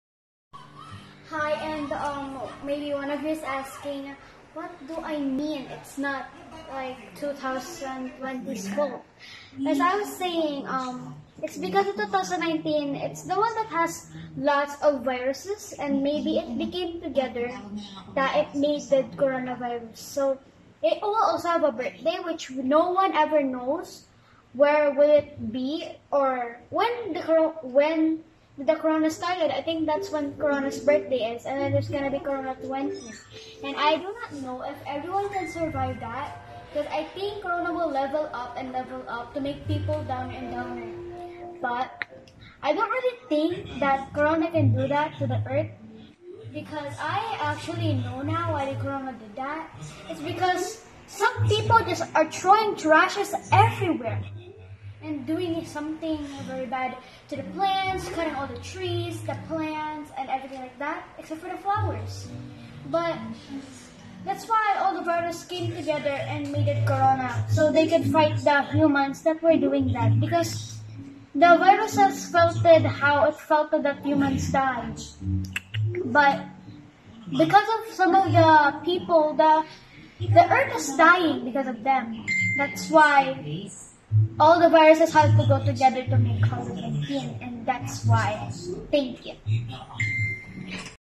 NOVEL CORONAVIRUS COVID - 19 SAAN NGA BA GALING??? Lets us hear it from 'AMAZING CHARM'
Amazing Charm 9 yr, daughter of my former officemate kuya Julius Bulan nagpahayag ng opinion ayun sa sanhi ng NOVEL CORONAVIRUS   COVID - 19. I was so amazed to this cute and very smart girl Amazing Charm.Personally I was toched and enjoying watching while she's talking and explaining what maybe the causes are this we covid - 19 .Hearing this kind of opinion from 9 year old girl such amazing thing ..I love you Charm thanks for sharing your thoughts to us🙏.